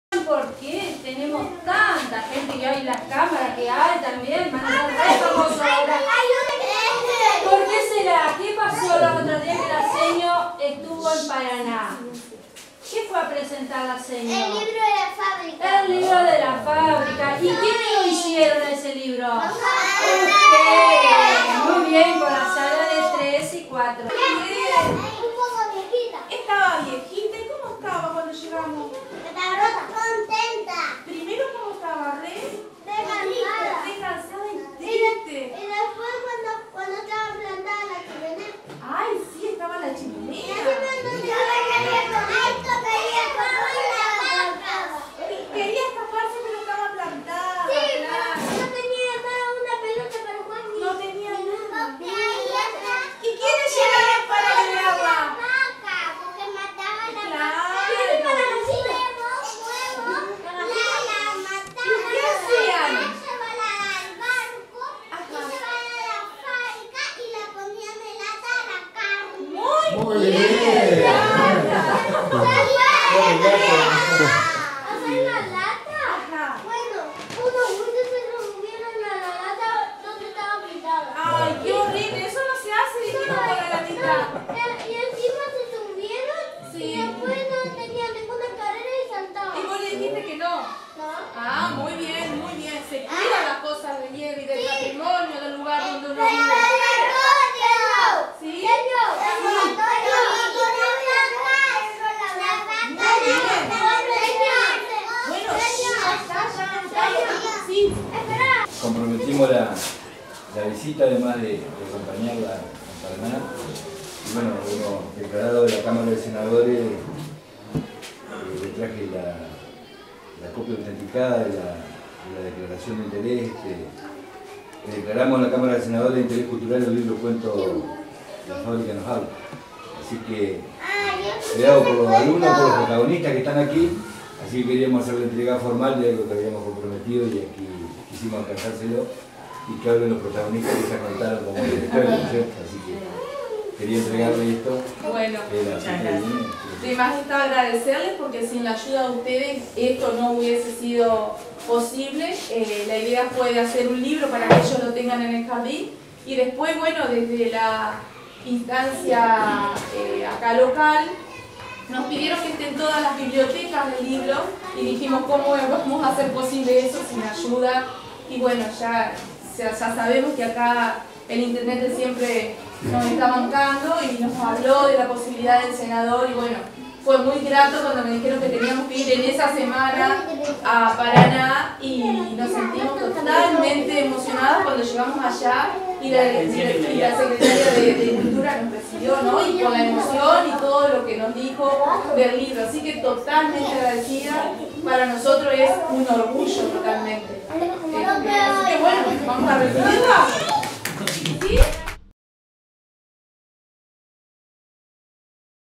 RECONOCIMIENTO POR LIBRO " LA FABRICA NOS HABLA "
ENTREGA DEL SENADOR SANTA CRUZ Y EL INTENDENTE PINTOS